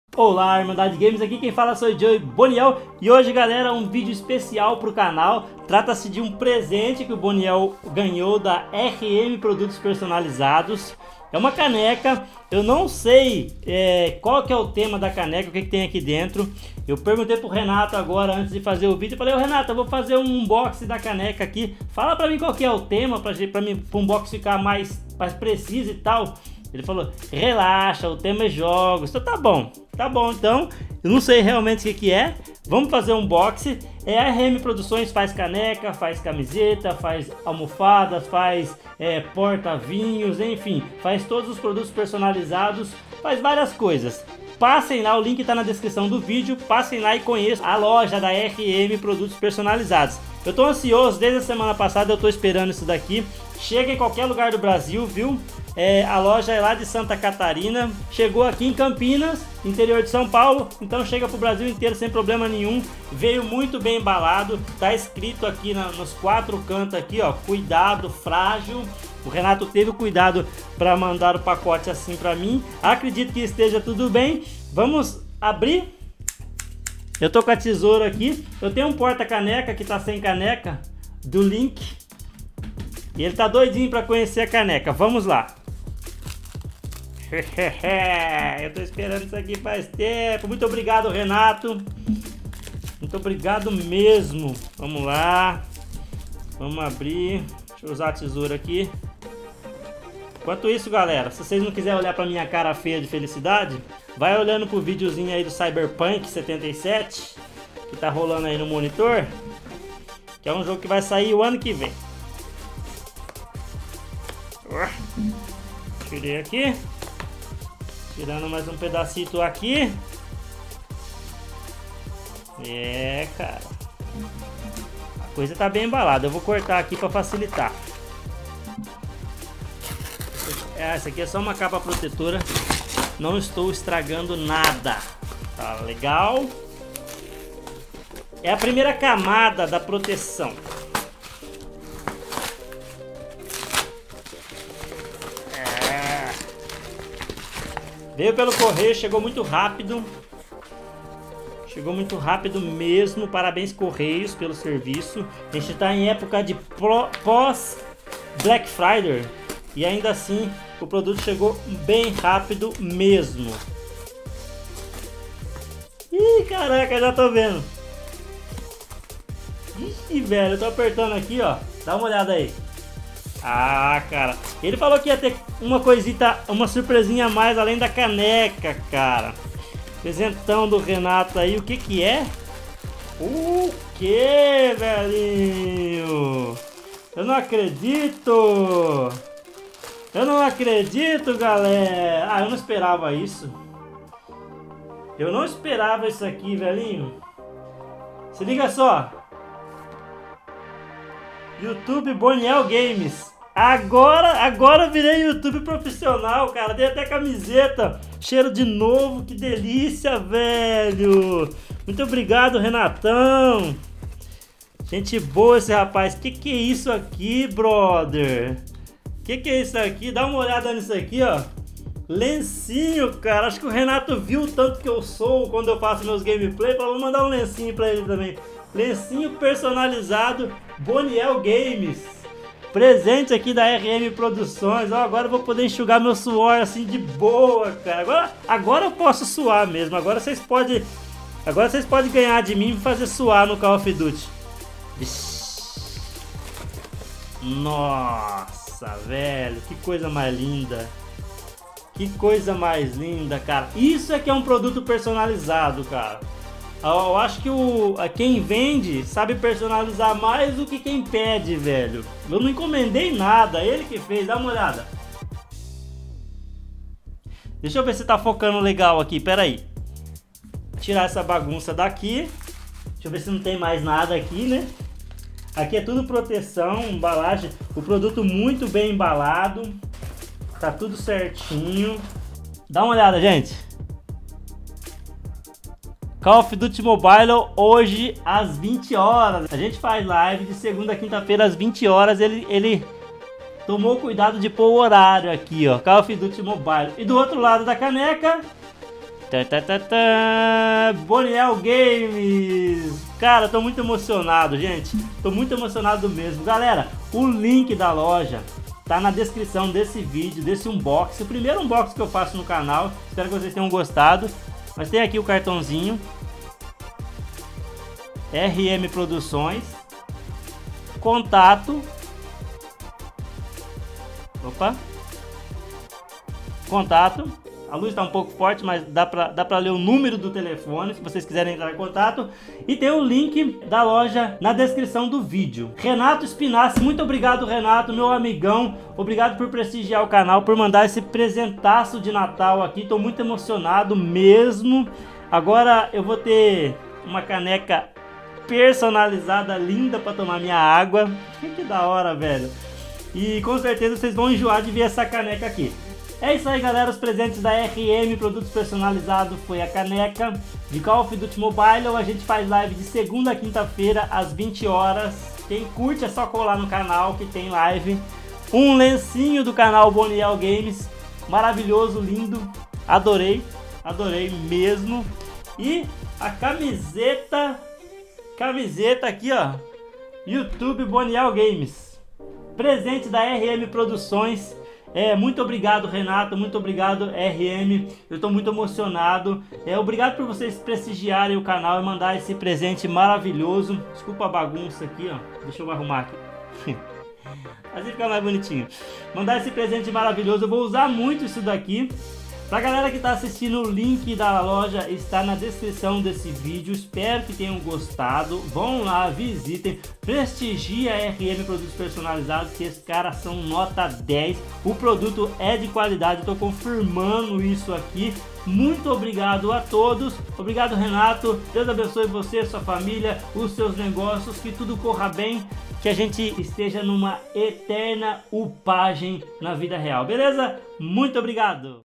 Unboxing surpresa de Caneca personalizada# Call of Duty Mobile com bonielgames
Unboxing da caneca de Call of Duty Mobile, presentinho de natal da loja R&M personalizados.
Muito Obrigado R&M!!!

Doe suas moedinhas para ajudar o canal a crescer!

Canais parceiros:

GAMER RABUGENTO call of duty cold war live de free fire call of duty warzone